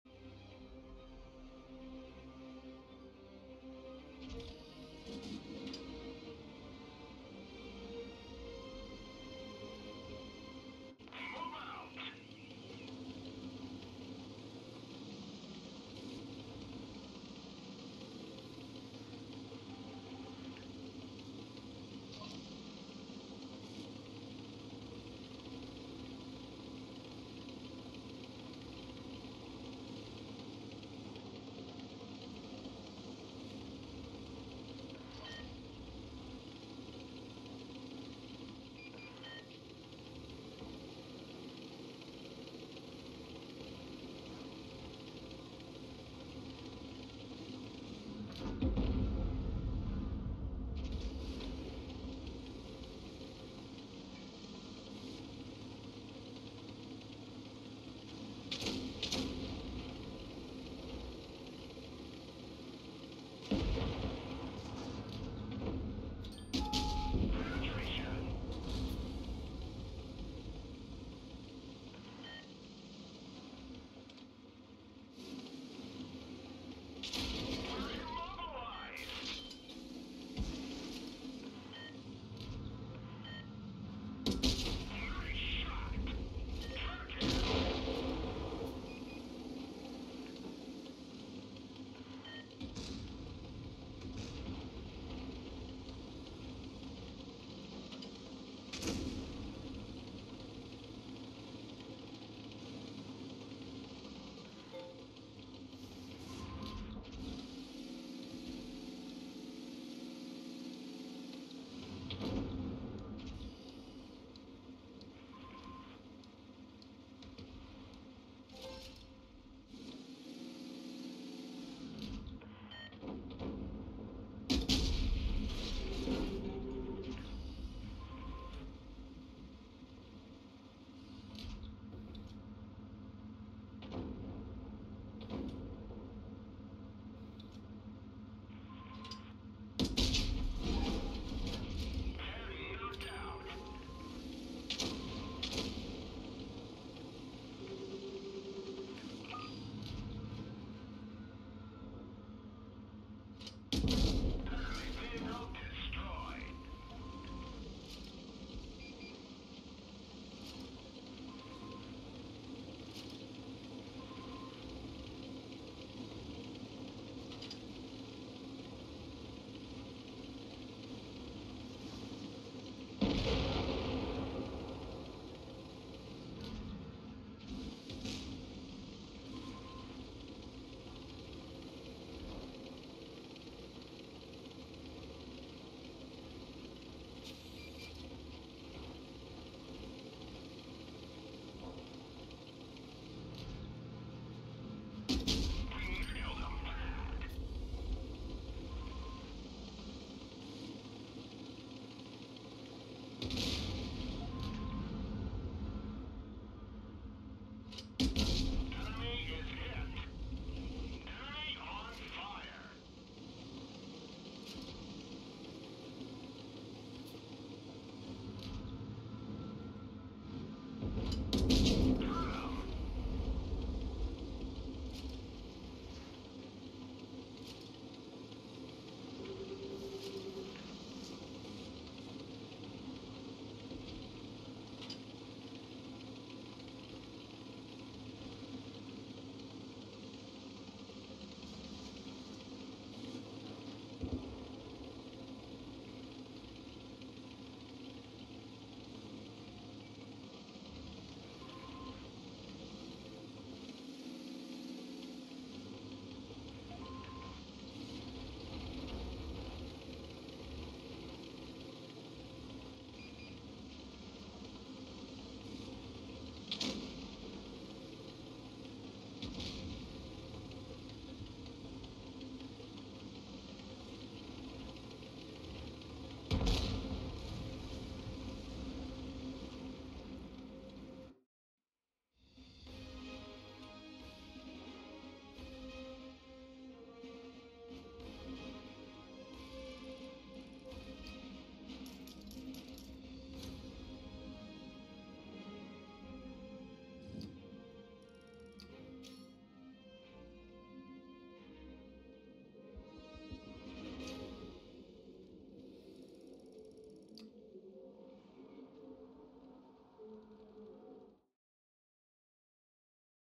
T30 | ACE Tanker #105
I only wish this tank was a tad faster.

Hope you enjoy this replay as much as I do.